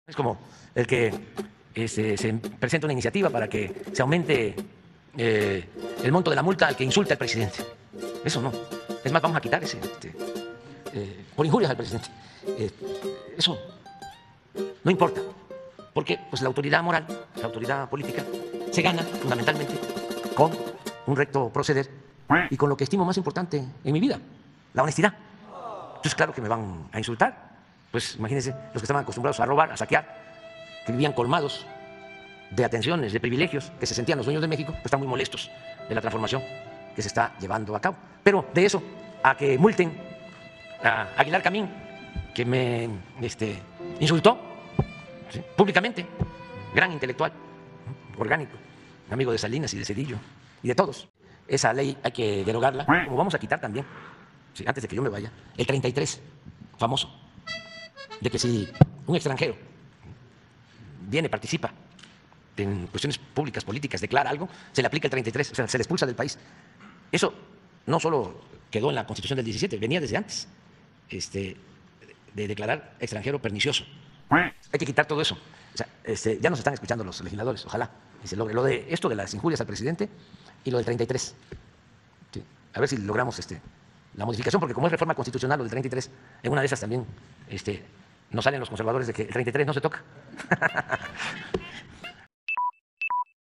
Vamos a derogar lo de multar por injurias al Pdte y el 33, ya me están escuchando los legisladores 🦆
Vamos a derogar lo de multar por injurias al presidente y el 33, ya me están escuchando los legisladores 🦆